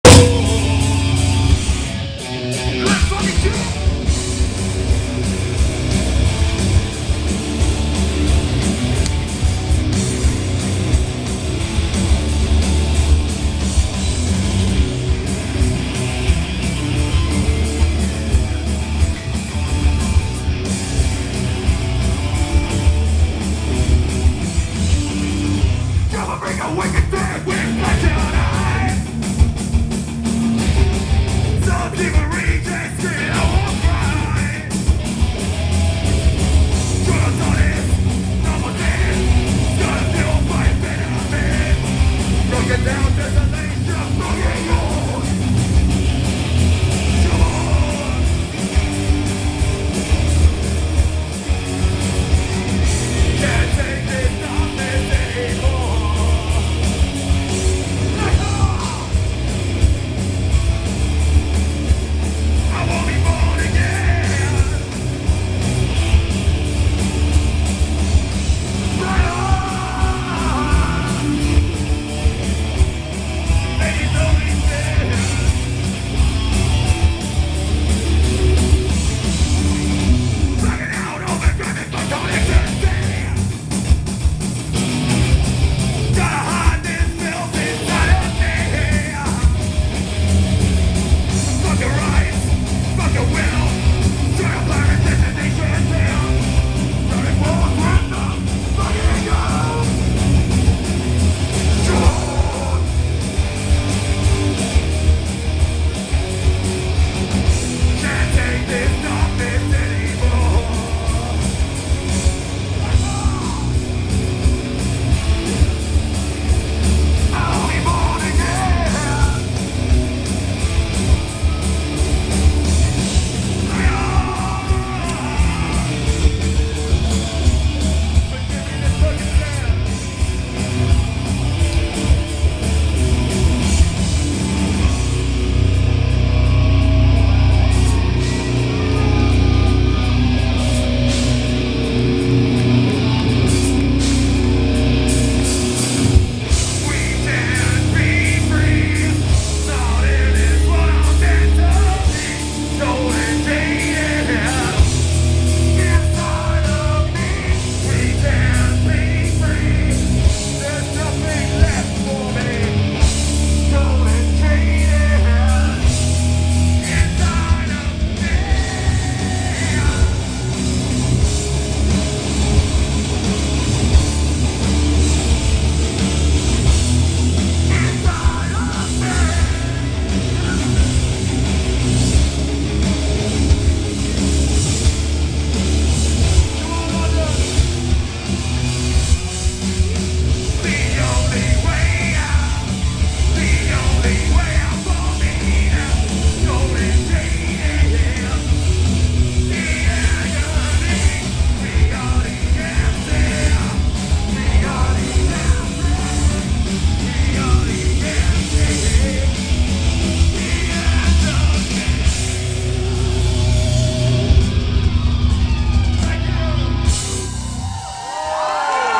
GENERATION GRAVEYARD@ISLINGTON ACADEMY LONDON 2013
april 3th london 2013 by choli
no copyrights intended
GENERATION GRAVEYARD as special guests
of mighty THE MISFITS